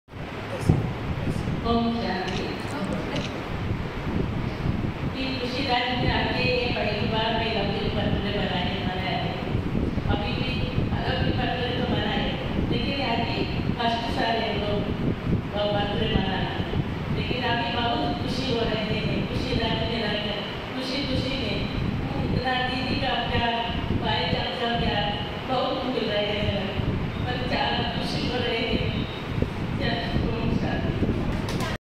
विजय लक्ष्मी माता खुशराज़ी भवन में बर्थडे मनाने पर
@CALalitInaniBK ऊषा नागपाल खुशराज़ी भवन में बर्थडे मनाने पर भाव विभोर होकर अपना अनुभव सुना रही है।